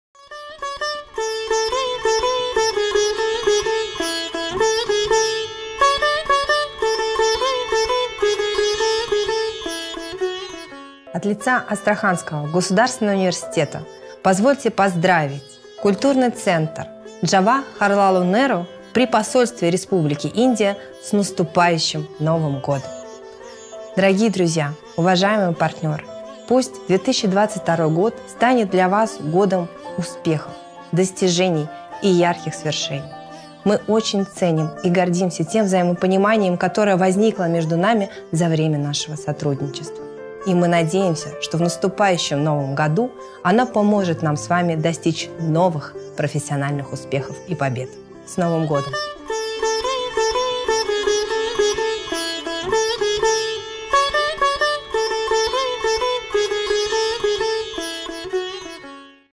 New Year messages from Russian friends of India
Отношения между Астраханью и Индией зародились в 16 веке, и эта дружба длится по сей день.

В канун Нового года проректор Астраханского государственного университета Ангелина Фадина желает всем успехов, достижений и ярких свершений в наступающем 2022 году.
Relations between Astrakhan and India originated in the 16th century, and till now this friendship continues.

On this New Year’s Eve, Vice-Rector of Astrakhan State University, Ms. Angelina Fadina, extends her best wishes of success, achievements and outstanding accomplishments for the upcoming 2022.